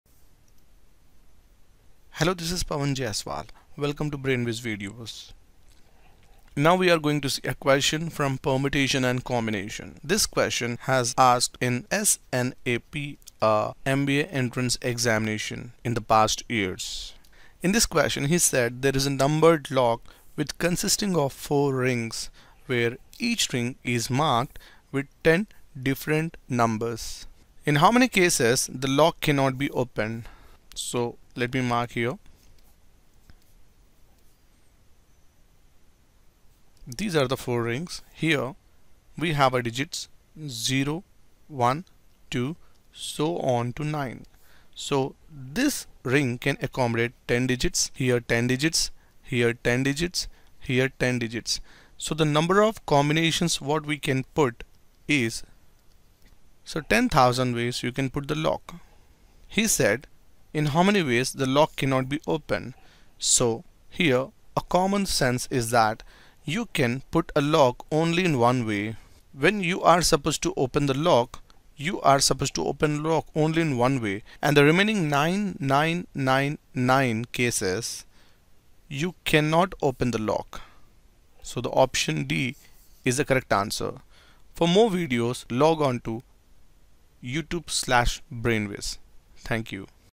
Permutation and Combination by Pavan Jaiswal | Basics, Methods & Tricks for SNAP
This question is asked in SNAP examinations

In this video is going to discuss Permutation and Combination - Basics, Methods, Shortcuts & Tricks for SNAP
Permutations and Combination is one section of the Quantitative Aptitude which needs the least number of formulae and the maximum amount of logic. This topic is asked in almost every competitive exam, so don’t take the risk of avoiding it.  After watching this lesson you will be familiar with Permutation and Combination.